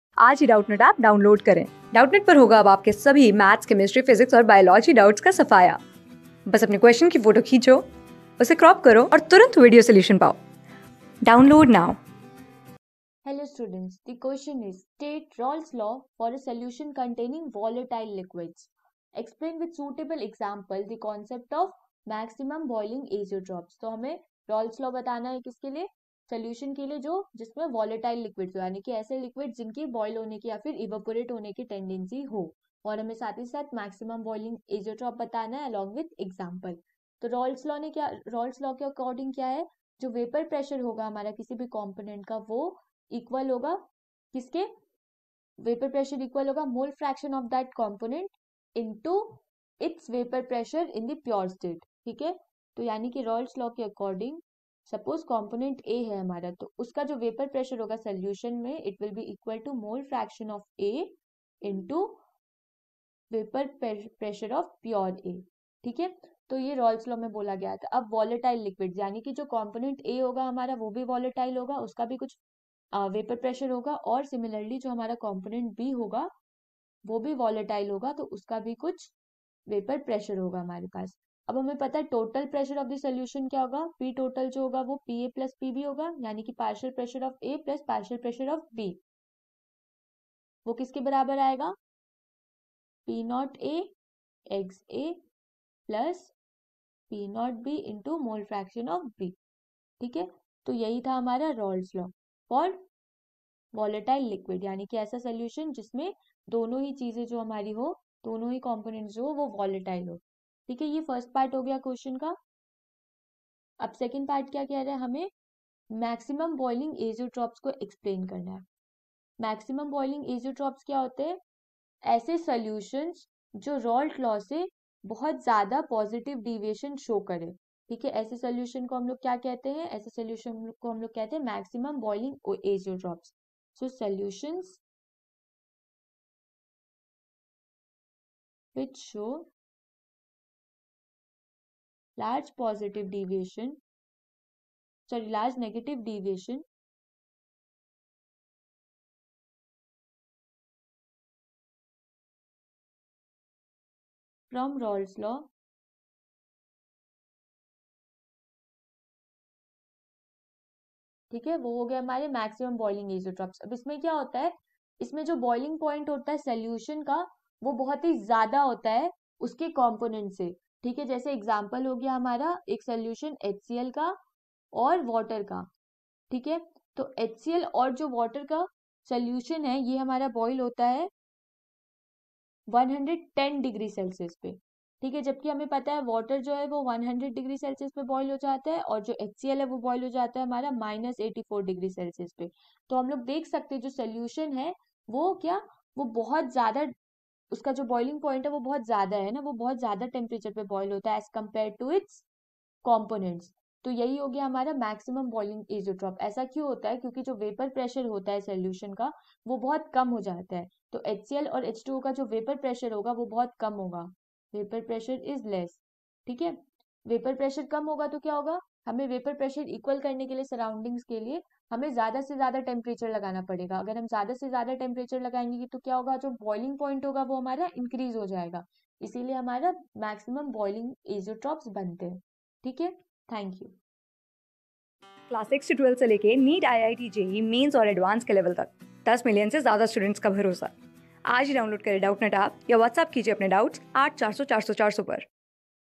State Raoult\'s law for a solution containing volatile liquids. Explain with suitable example th...
State Raoult\'s law for a solution containing volatile liquids. Explain with suitable example the concept of maximum boiling azeotropes.
Class: 12
Subject: CHEMISTRY
Chapter: SOLUTIONS
Board:CBSE

👉 Have Any Query? Ask Us.